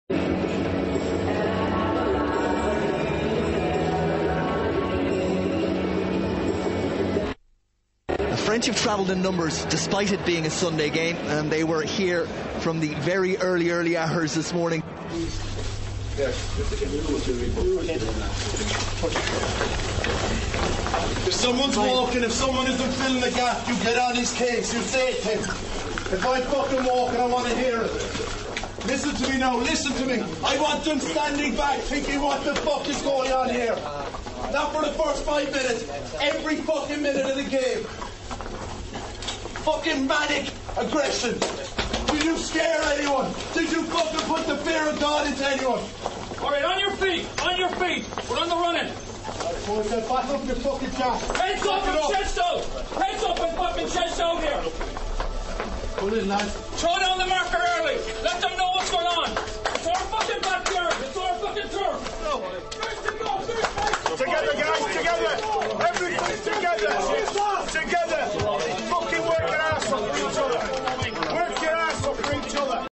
Paul O'Connell - Manic Aggression
Ireland France Rugby Six Nations 2007, pre-game dressing room footage. Paul O'Connell (Lions Captain) puts the fear of God into his team mates